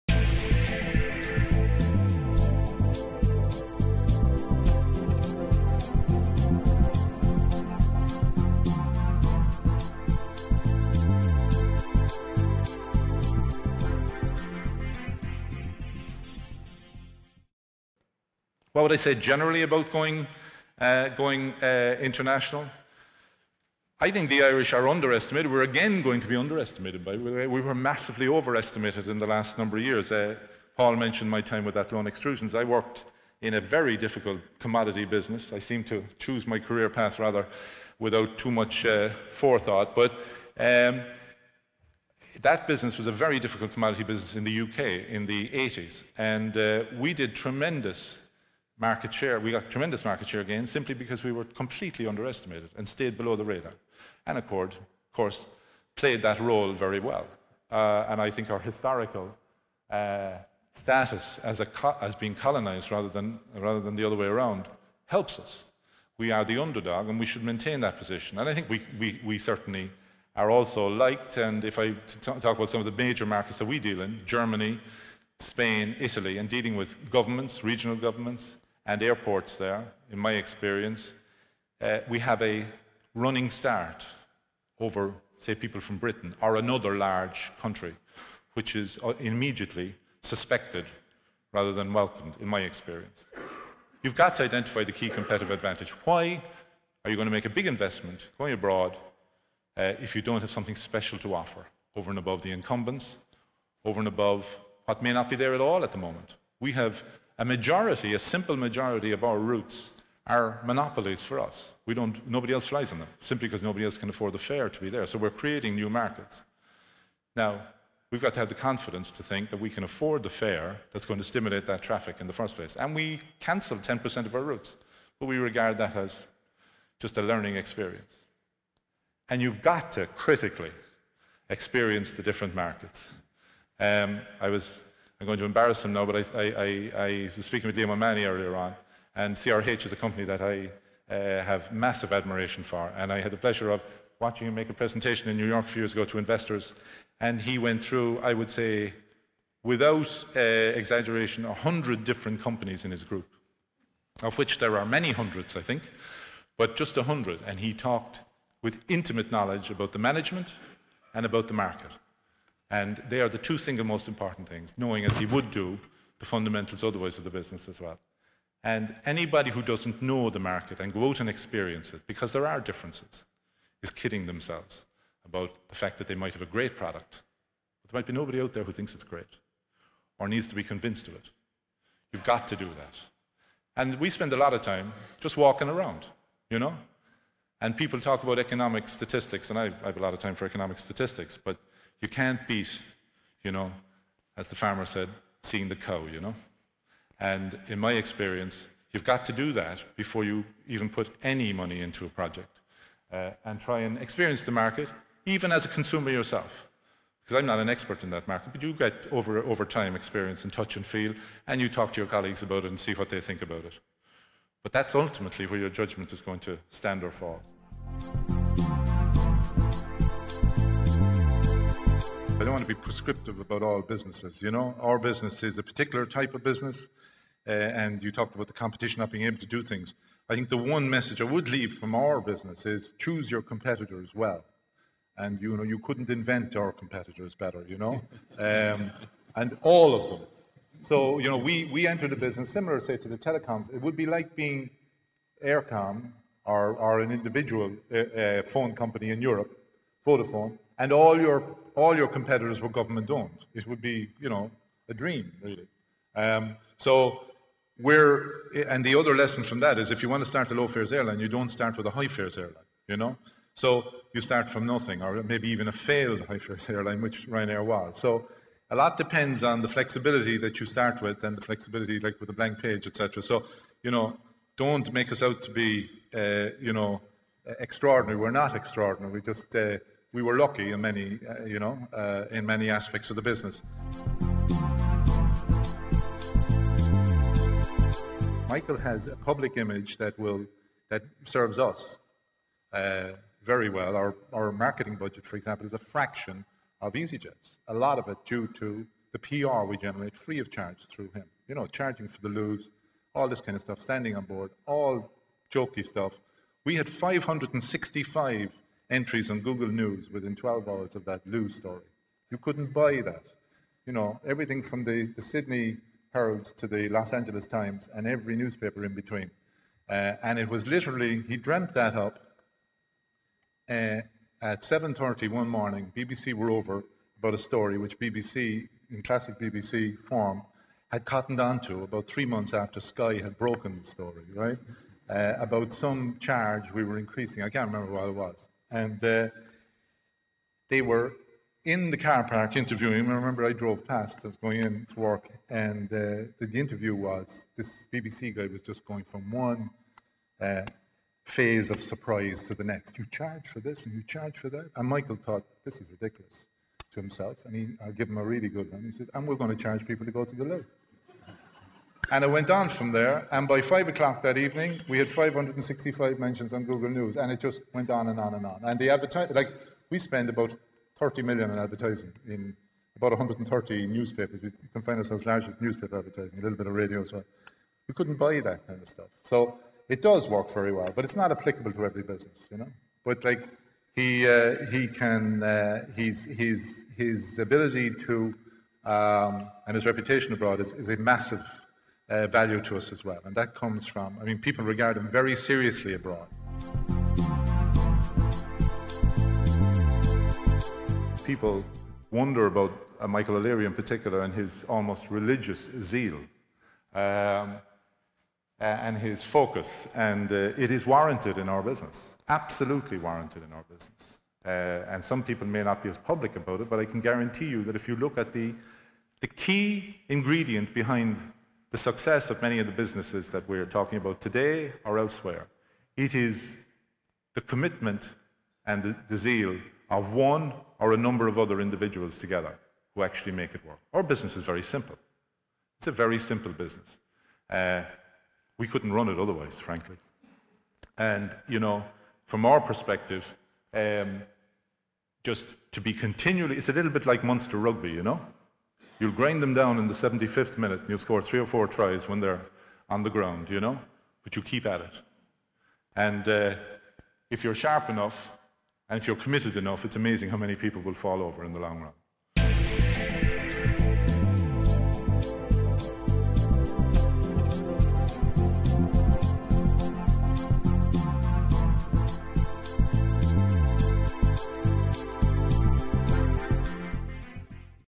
Beyond Borders: Commitment and zeal in the leadership team
Extended highlights from the keynote presentation given by Michael Cawley, Deputy CEO of Ryanair, including the inside track on the famous paying for the loo incident, at the Beyond Borders event hosted by UCD Michael Smurfit Graduate School of Business, to discuss the internationalisation of Ireland's businesses in November.